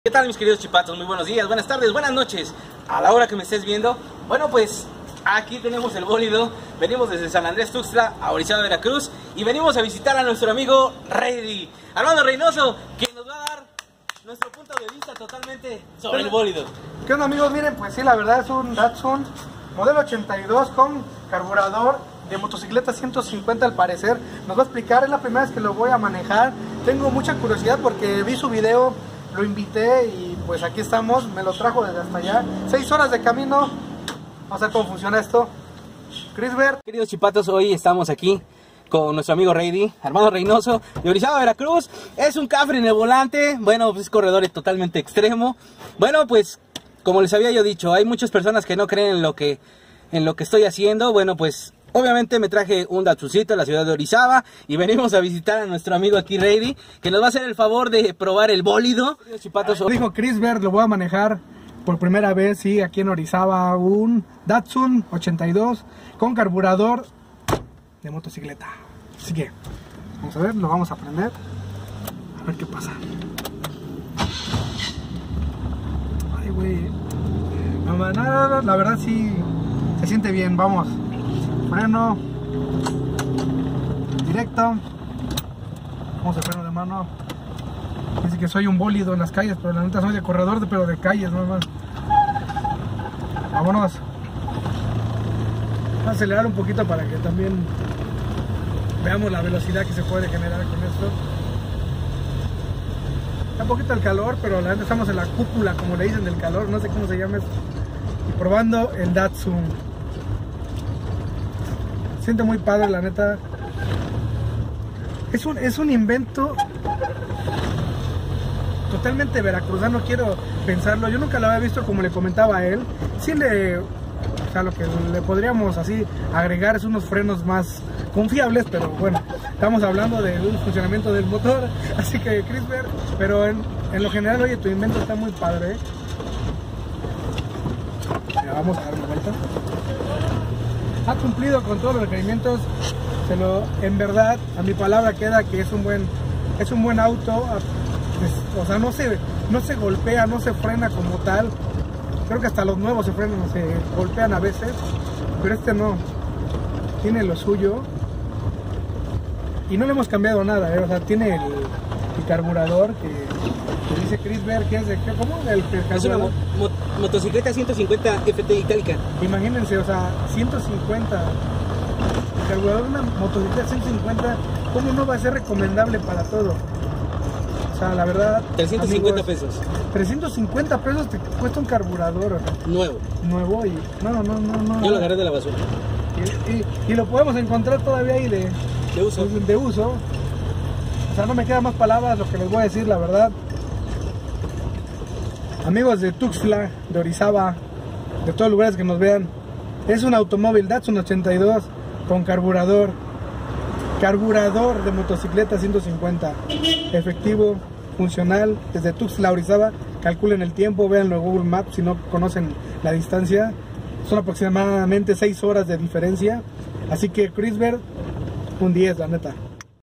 PROBANDO INVENTO DE CARRO CON CARBURADOR DE MOTO 150CC ITALIKA
Les presentamos la prueba de que el auto datsun modelo 82 llego a orizaba con el carburador 150 cc italika, la prueba a cargo de ARMANDO BROOW DEL